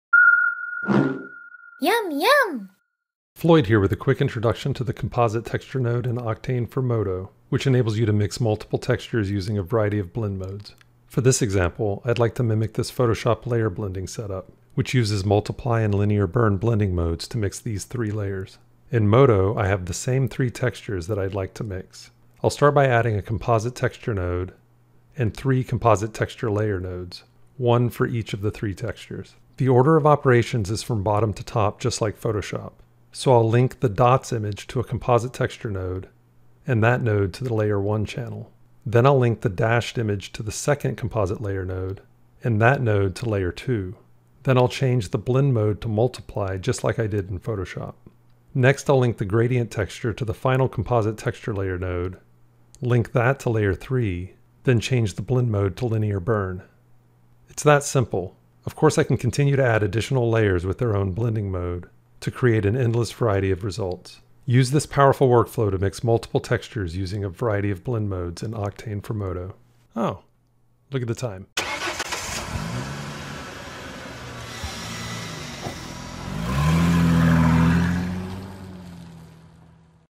Modo | Octane Render: Composite Texture Node
This video shares a quick introduction to the Composite Texture Node in Octane for Modo, which enables you to mix multiple textures using a variety of blend modes.